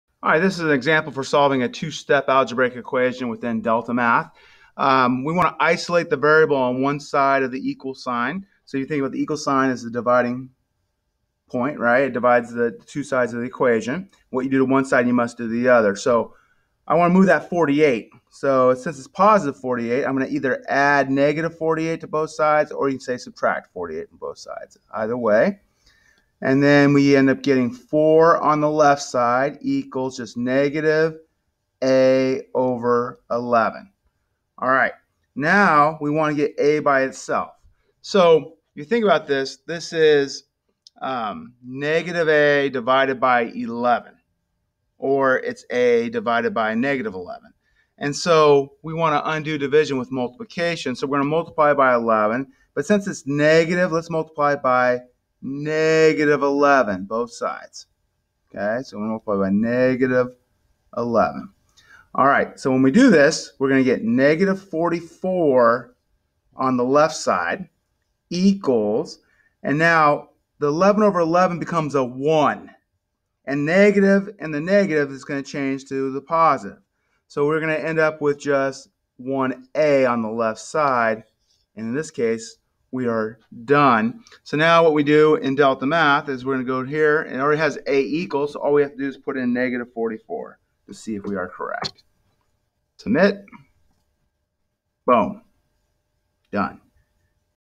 DeltaMath: Solve Two Step Equations
This video provides one example for solving a two step Algebraic Equation within the DeltaMath online system.